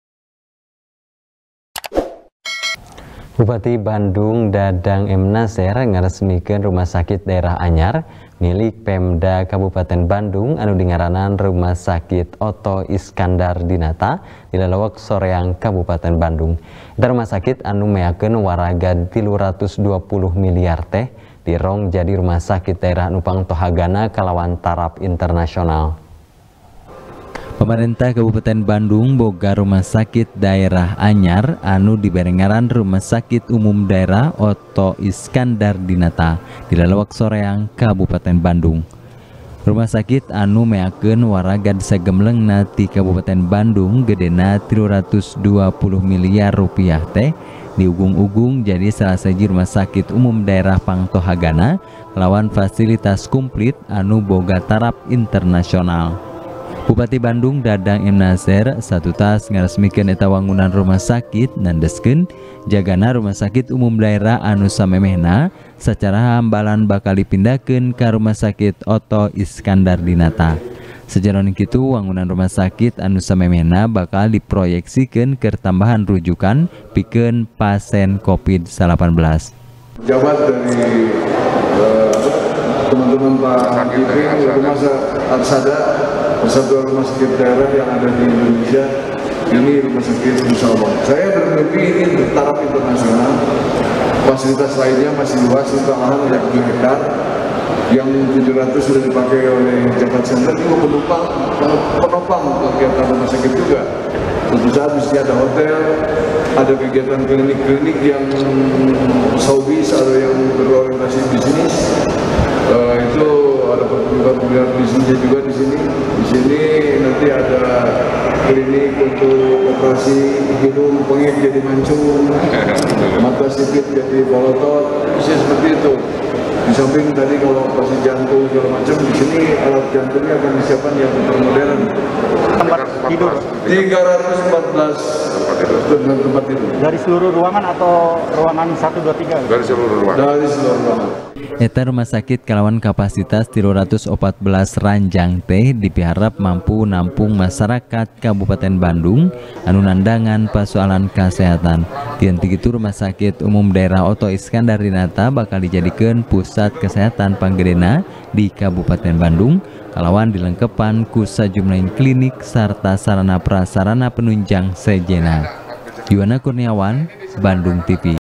Pemkab Bandung Boga Rumah Sakit Taraf Internasional | TP | BANDUNGTV
TP | BANDUNGTV | IWAN M RIDWAN | Bupati Bandung Dadang M. Naseer Ngaresmikeun Rumah Sakit Daerah Anyar Milik Pemda Kabupaten Bandung, Anu Dingaranana Rumah Sakit Otto Iskandar Dinata, Di Lelewek Soreang Kabupaten Bandung. Eta Rumah Sakit Anu Meakeun Waragad Tilu Ratus Dua Puluh Miliyar Teh, Dierong Jadi Rumah Sakit Daerah Anu Pangtohagana Kalawan Taraf Internasional.